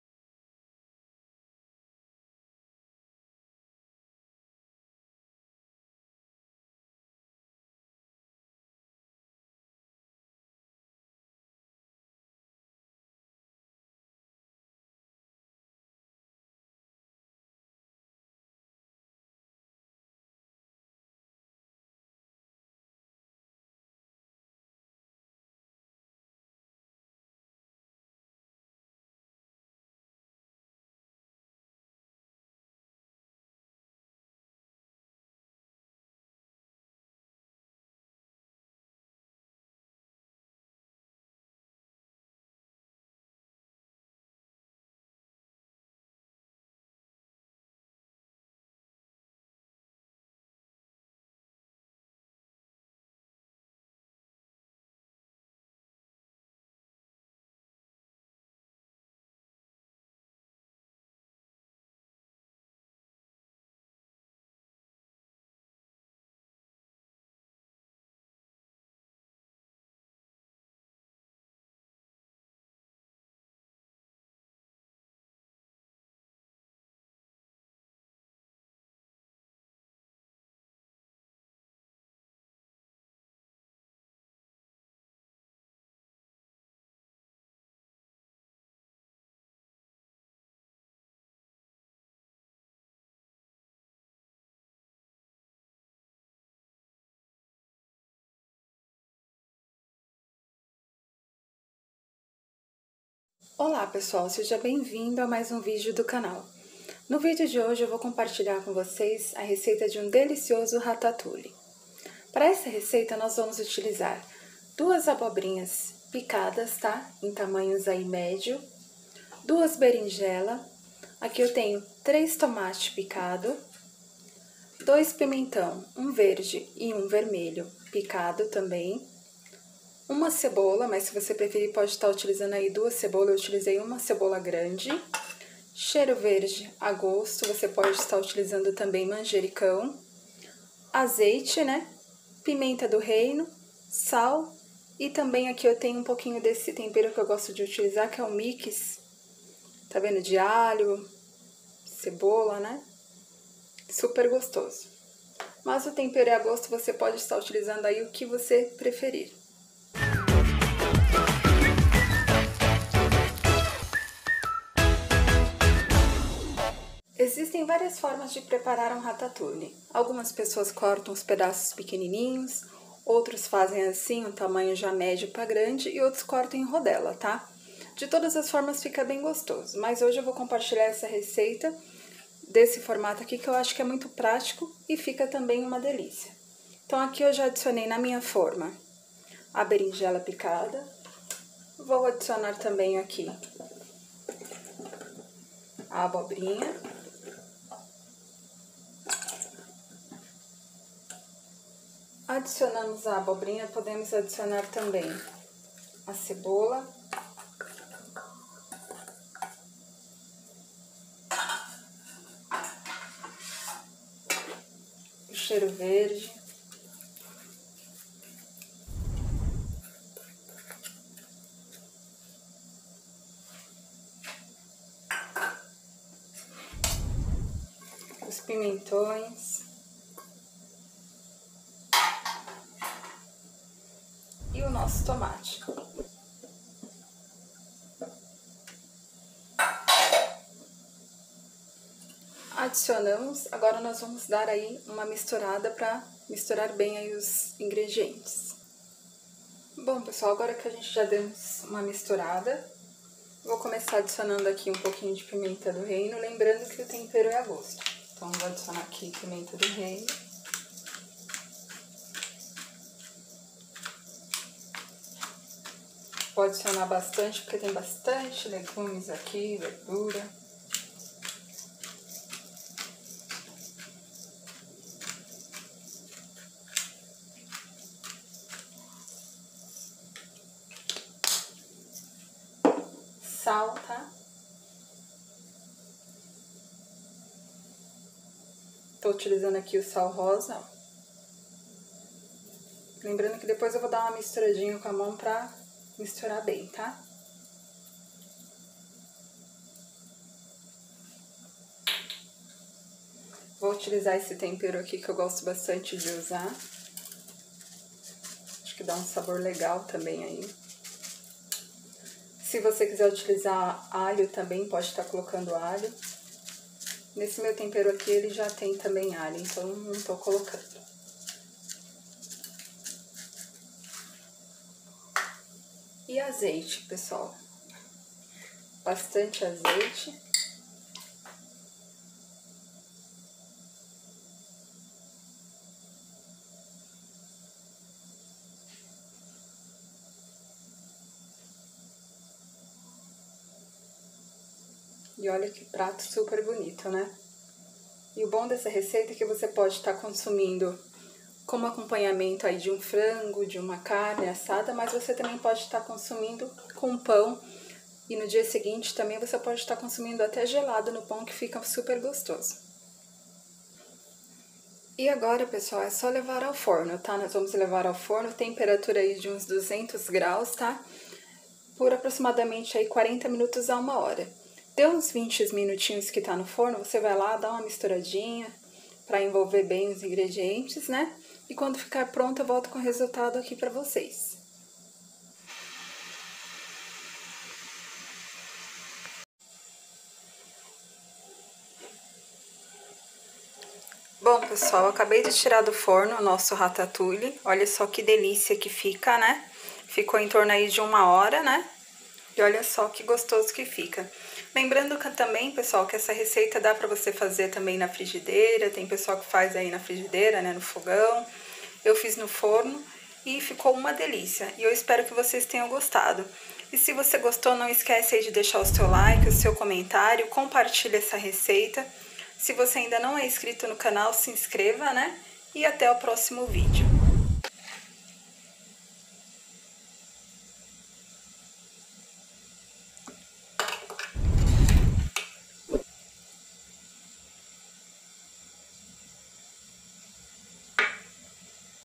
RATATOUILLE, RECEITA PRÁTICA E DELICIOSA
Olá pessoal! Sejam bem vindo a mais um vídeo do canal.

E hoje eu quero compartilhar a Receita de RATATOUILLE.

Essa receita é uma delícia,e fica um cheiro maravilhoso pela casa...Ah! e ela pode ser consumida quente ou fria, como acompanhamentos de carnes e também pode consumir com pão 😋

☑️ INGREDIENTES:
2 Berinjela
2 Abobrinha
3 Tomate
1 Cebola grande
Pimentão vermelho e verde
Azeite
📌 Assista ao preparo no vídeo

😉 Compartilhe essa receita
Ajude nossa canal crescer!

🎥Visite o canal do meu esposo
PALAVRA DE FÉ
Lá tem sempre uma Palavra de Deus para sua vida 🙏🏽